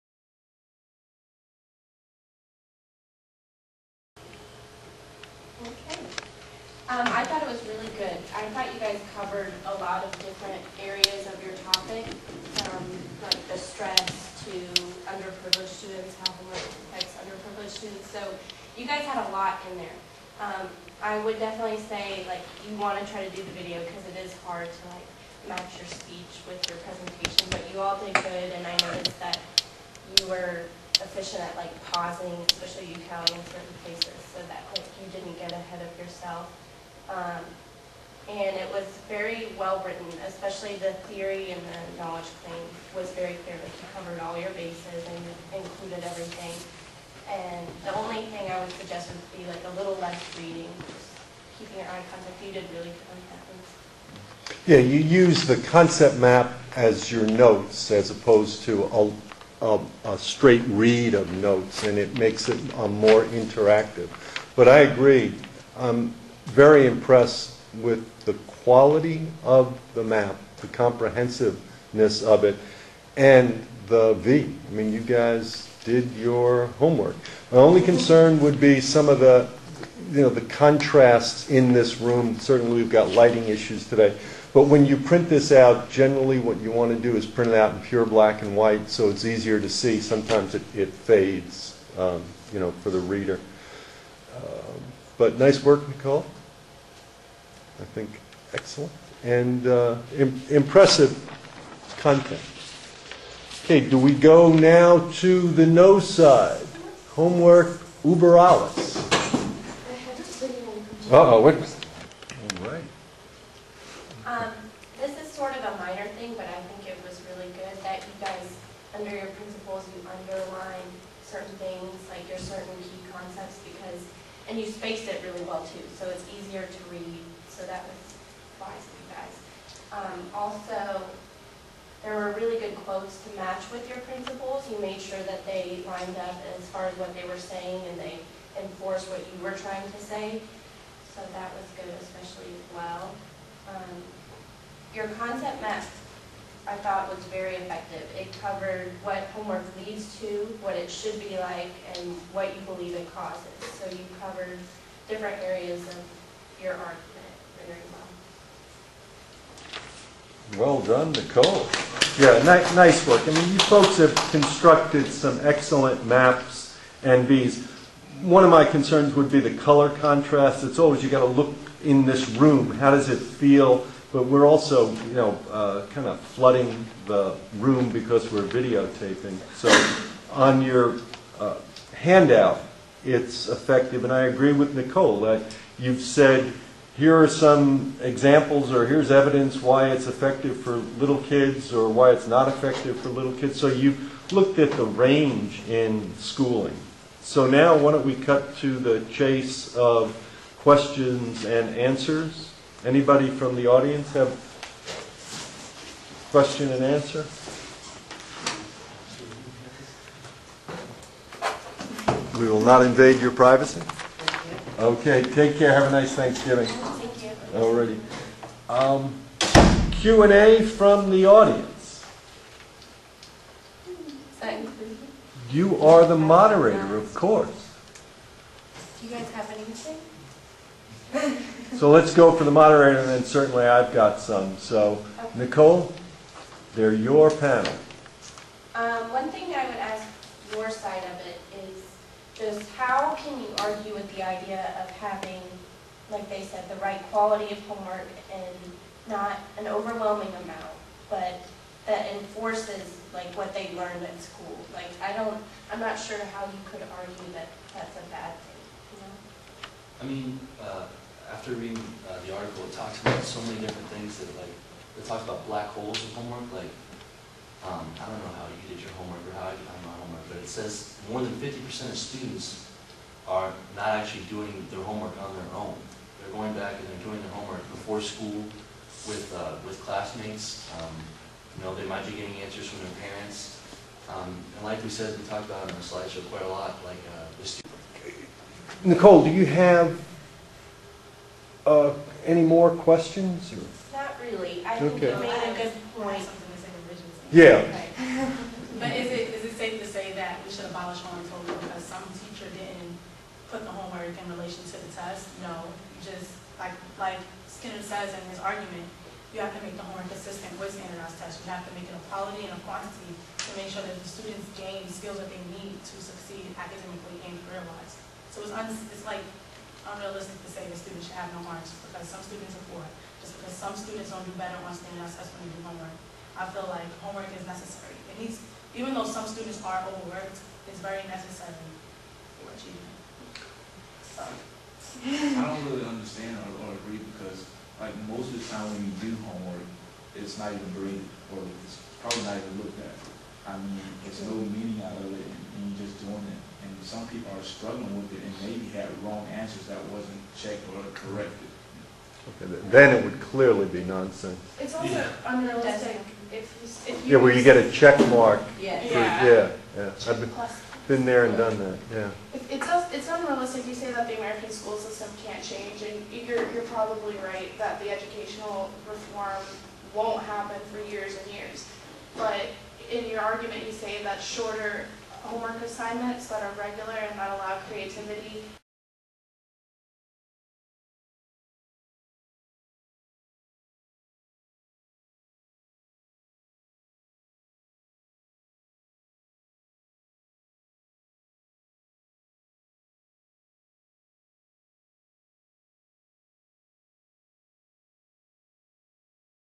Nicole Moderates a Panel1.wmv
Nicole moderates a panel presentation in Dr. Robert Leahy's introductory Social Foundations of Education course at Stetson University. She offers feedback to fellow students and involves them in a Q&A format to discuss their views about homework based on articles from James Noll's Taking Sides: Clashing Views on Educational Issues.